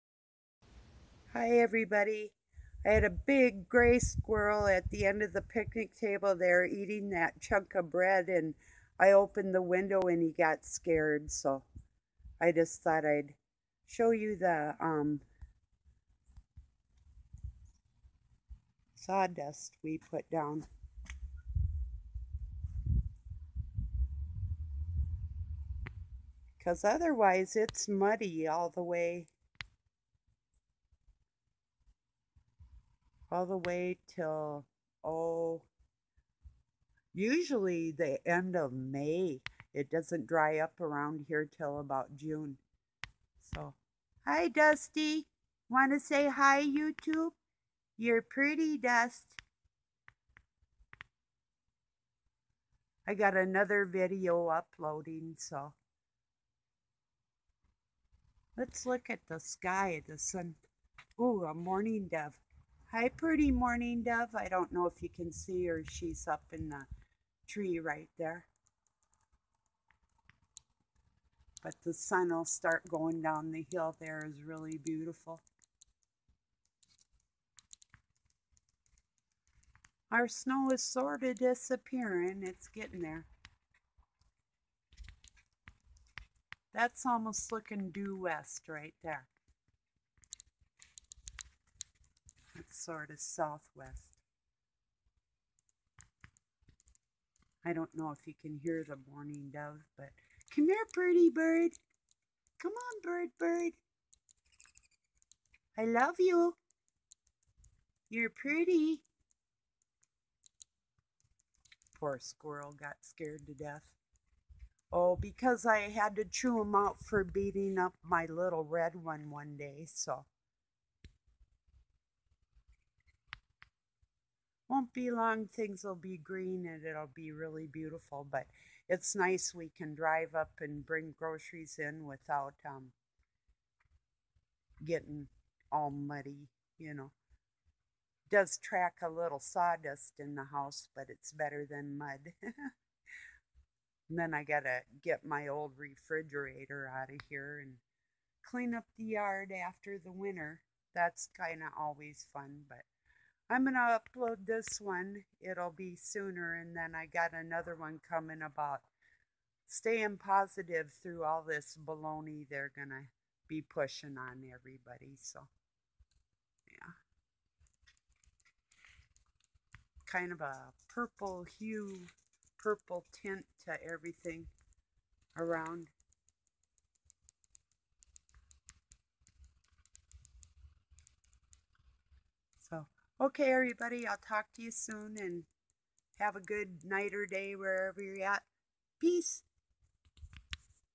Spring is trying to get sprung 😀🌼🌷✌🙋
Morning Doves , squrels and Dusty  🕊️🐿️🐎 .